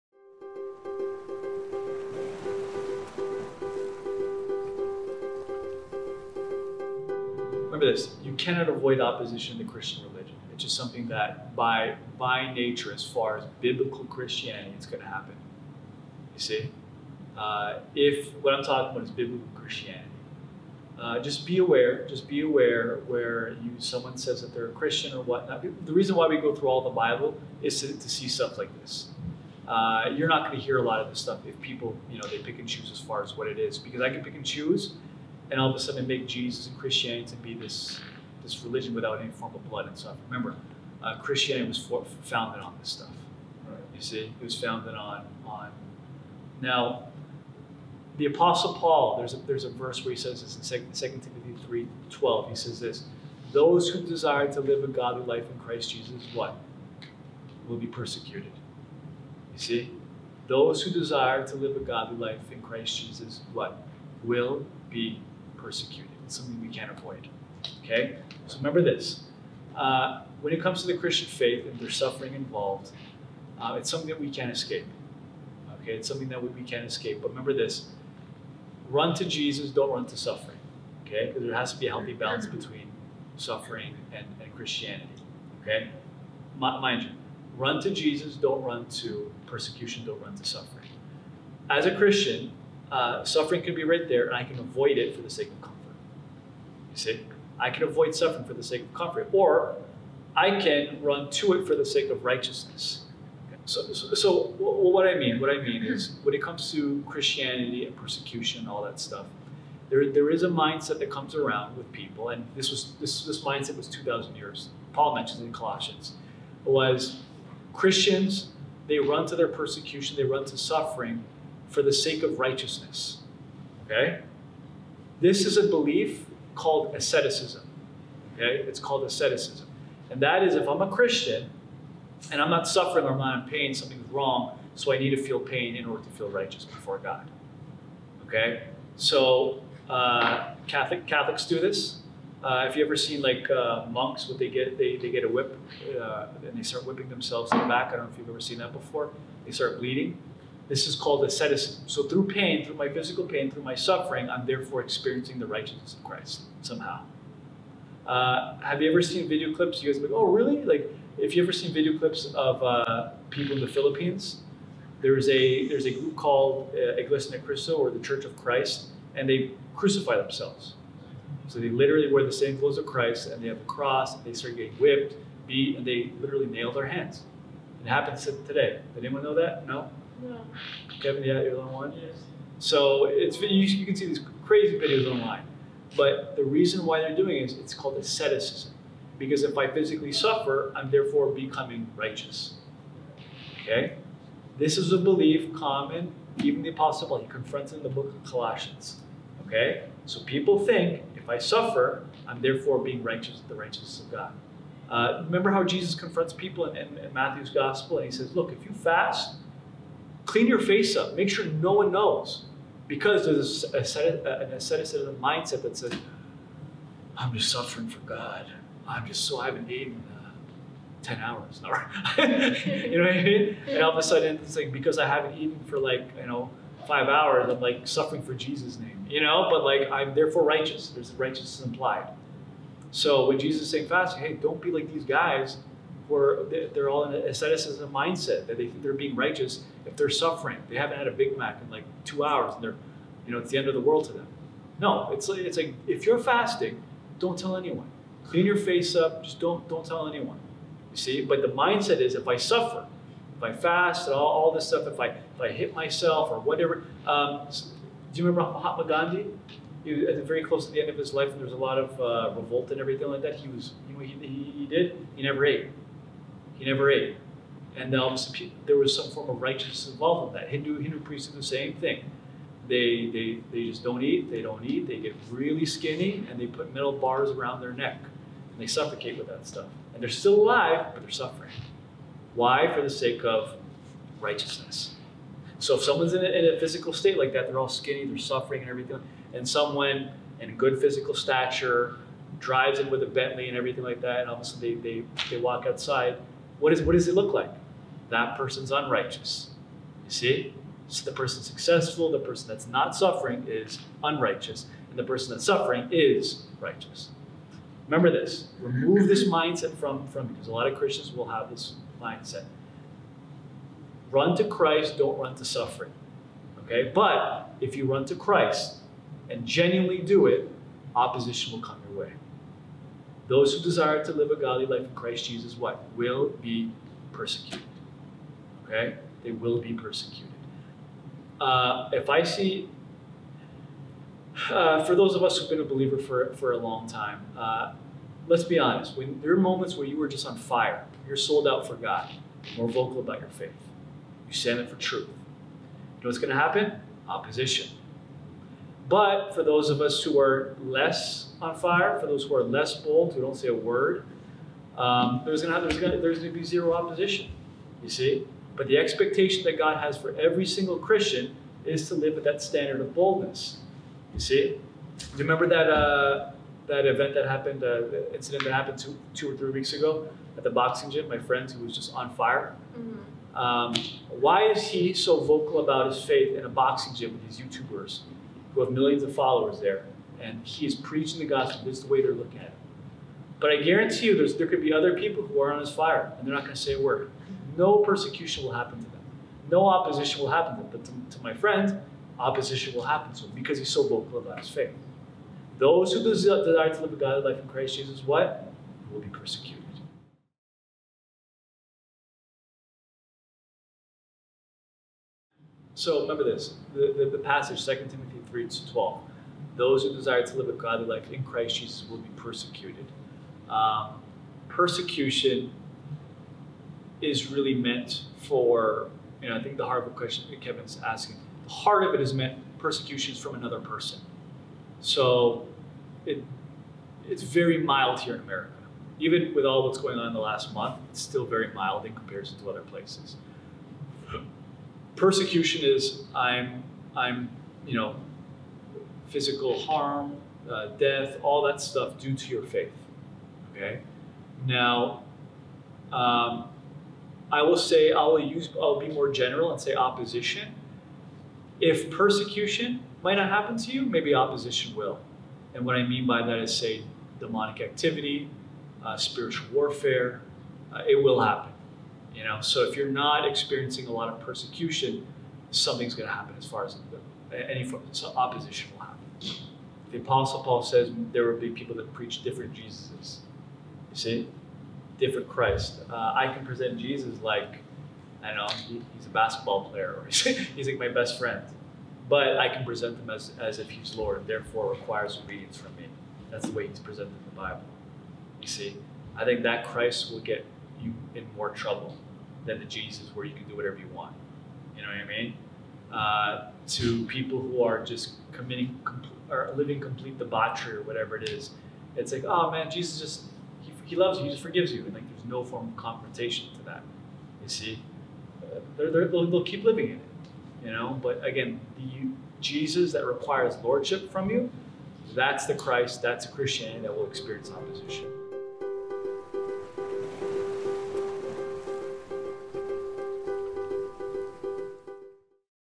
No Christianity Without Persecution
Pastor Reuben Carles speaks about how Christians throughout time have face persecution and oppression.

Come worship with us at our weekly bible study
every Thursday @ 6PM (Mask or face covering required)
223 W Franklin Ave

Stay Encouraged and God Bless!